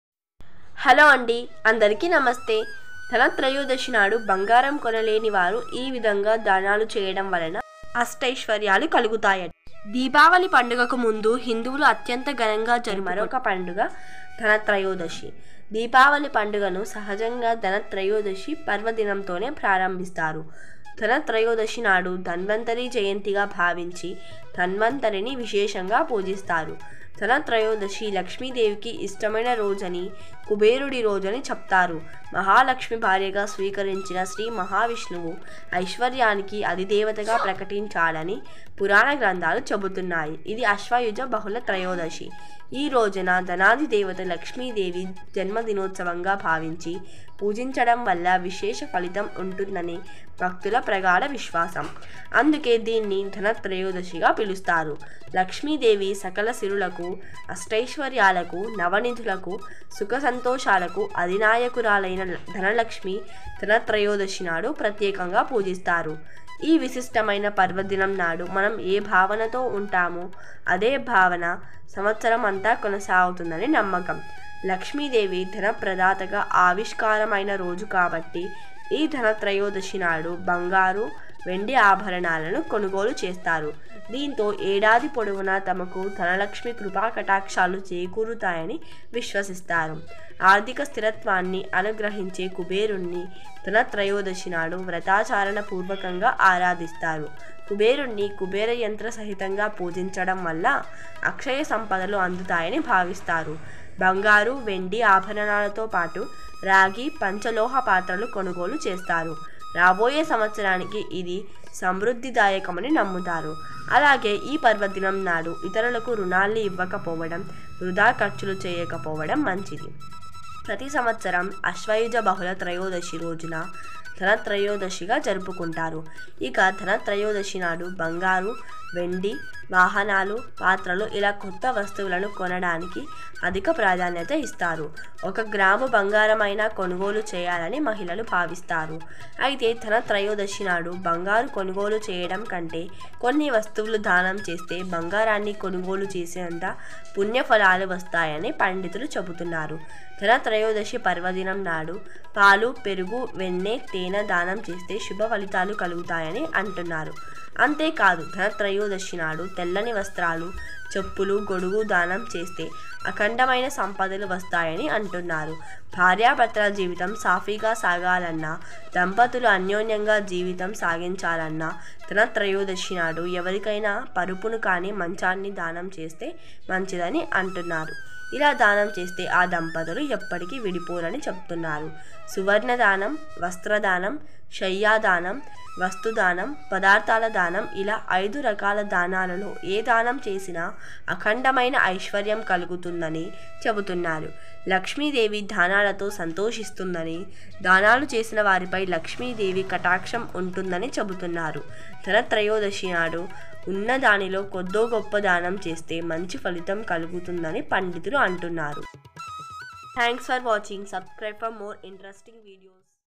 ధనత్రయోదశి నాడు బంగారం కొనలేనివారు ఈ విధంగా దానాలు చేయడం వలన అష్టైశ్వర్యాలు కలుగుతాయట...!!
Deepavali ధనత్రయోదశి నాడు బంగారం కొనలేనివారు ఈ విధంగా   చేయడం వలన అష్టైశ్వర్యాలు కలుగుతాయట...!!